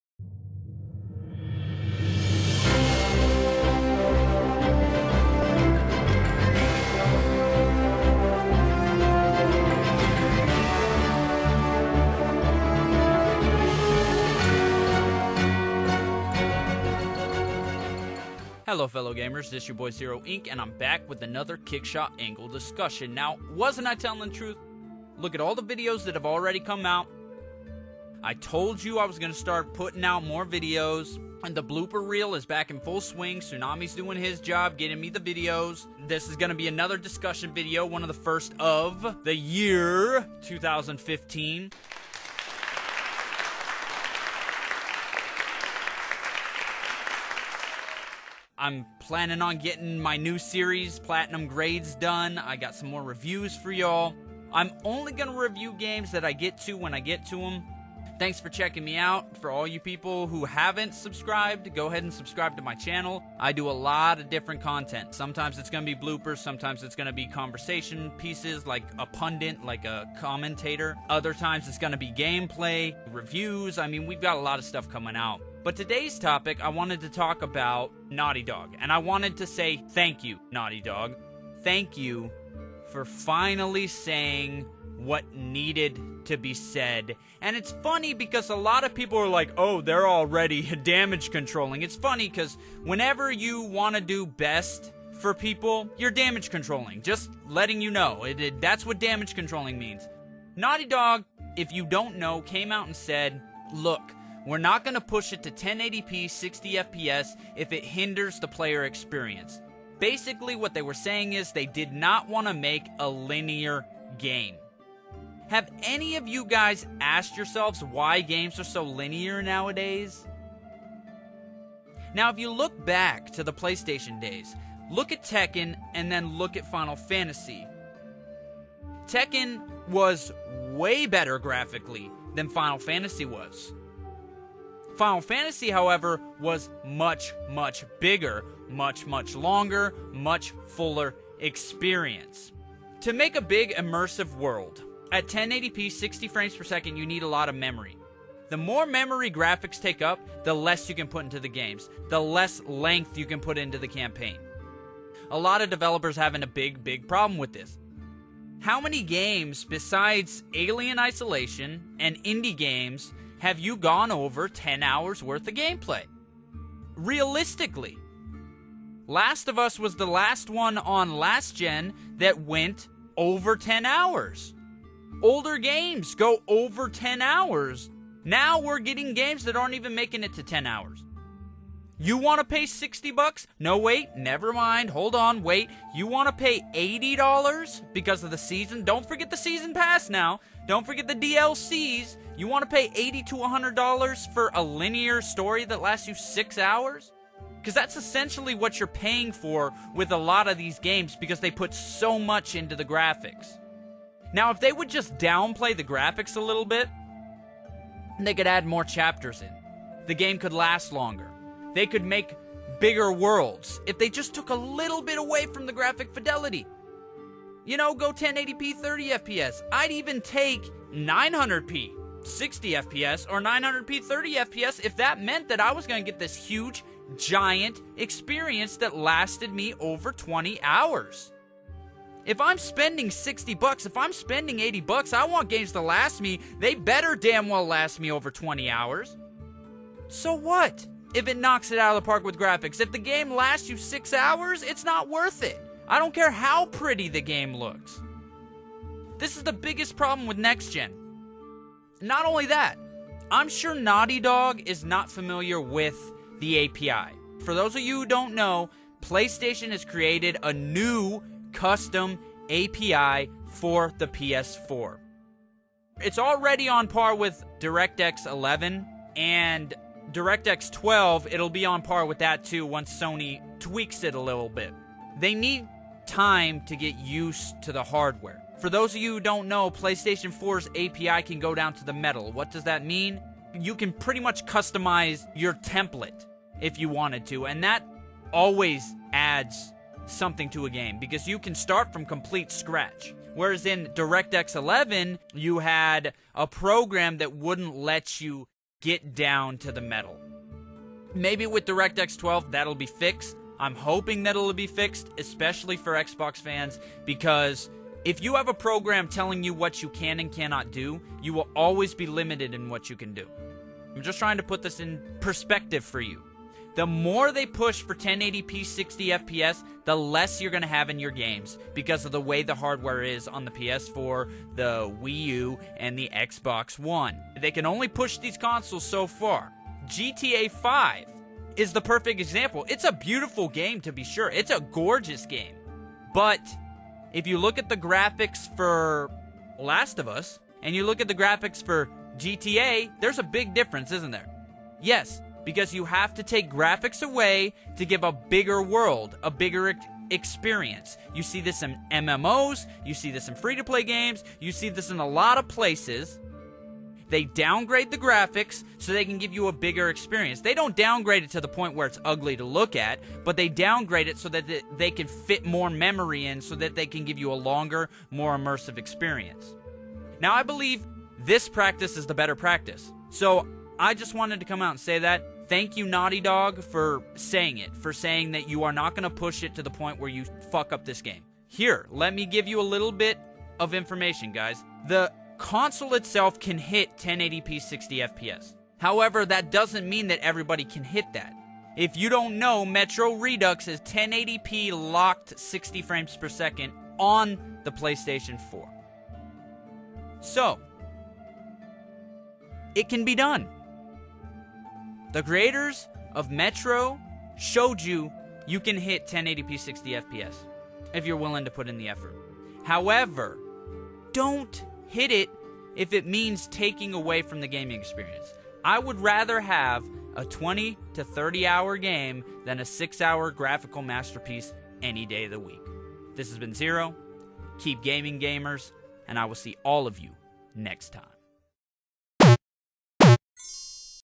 Thank You Naughty Dog For Doing Whats Best!
Just my thoughts on Naughty Dog saying they won't push the resolution or frame-rate if it hinders the players experience!! Thank you Naughty Dog for doing the right thing!!!